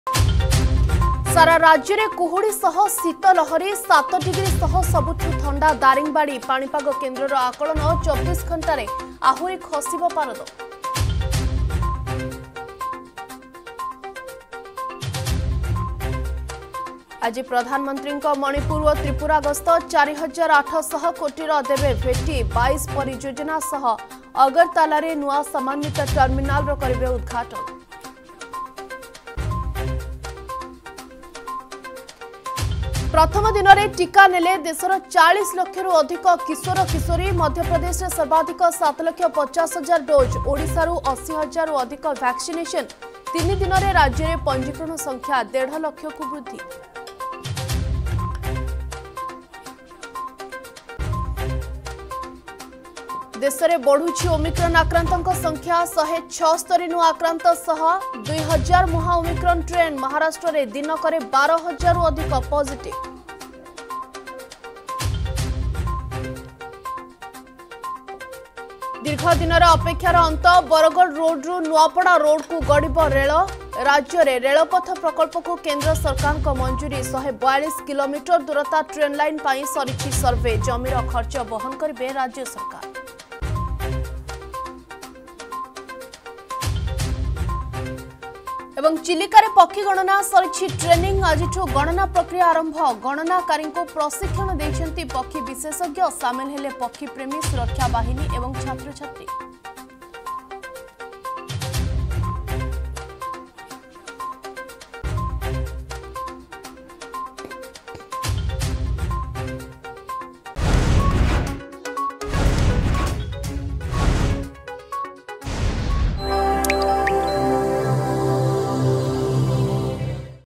Headlines 7 am | 4 Jan 2022 | Argus News
+ସାରା ରାଜ୍ୟରେ କୁହୁଡ଼ି ସହ ଶୀତ ଲହରୀ..
+ଆଜି ପ୍ରଧାନମନ୍ତ୍ରୀଙ୍କ ମଣିପୁର ଓ ତ୍ରିପୁରା ଗସ୍ତ...
+ପ୍ରଥମ ଦିନରେ ଟିକା ନେଲେ ଦେଶର 40ଲକ୍ଷରୁ ଅଧିକ କିଶୋର କିଶୋରୀ...
+ଦେଶରେ ବଢ଼ୁଛି ଓମିକ୍ରନ ଆକ୍ରାନ୍ତଙ୍କ ସଂଖ୍ୟା ..
+ଦୀର୍ଘ ଦିନର ଅପେକ୍ଷାର ଅନ୍ତ...
+ଚିଲିକାରେ ପକ୍ଷୀ ଗଣନା..